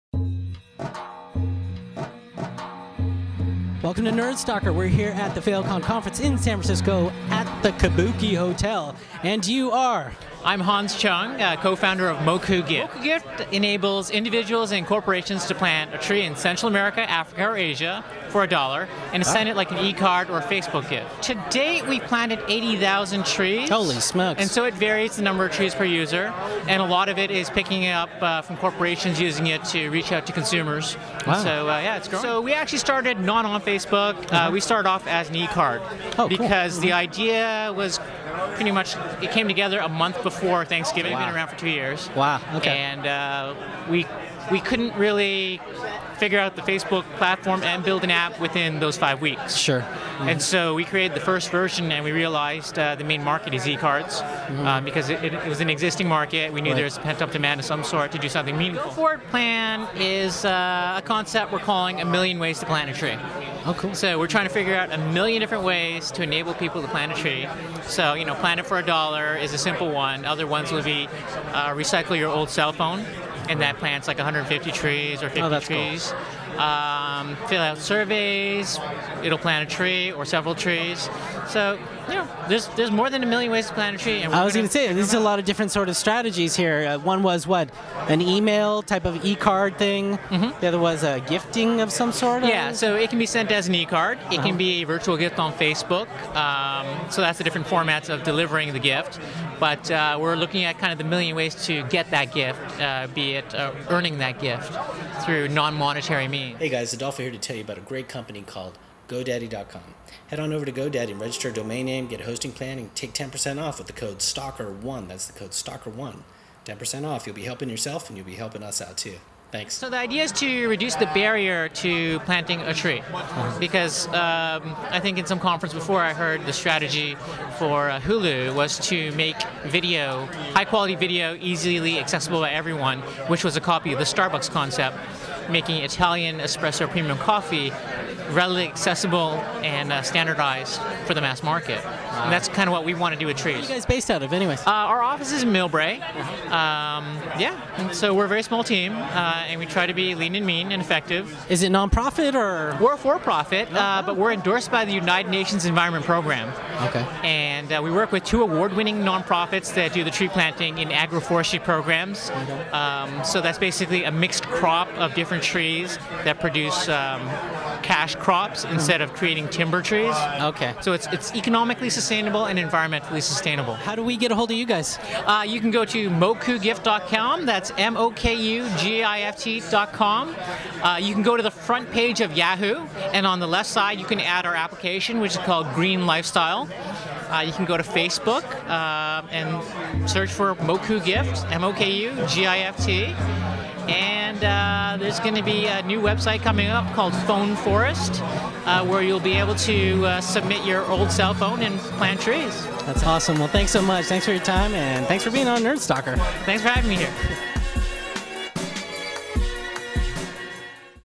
Want To Help Plant 1 Million Trees Online?
Nerd Stalker interview with Mokugift co-founder Hans Chung at a recent Failcon conference in San Francisco.  Distributed by Tubemogul.